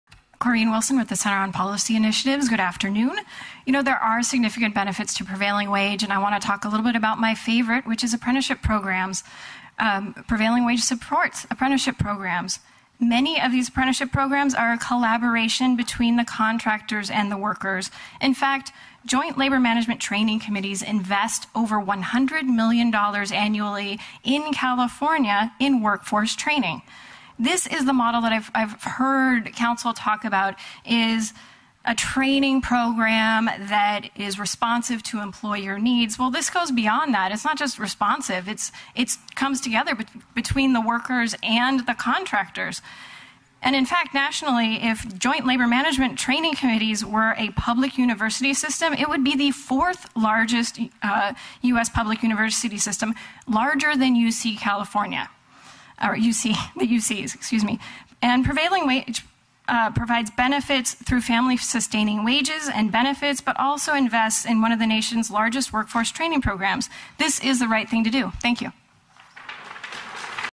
Research and policy analysts Corinne Wilson on the prevailing wage (720p)
On July 30, 2013 San Diego City Council passed prevailing wage. CPI's research and policy analyst Corinne Wilson provided testimony in support of prevailing wage.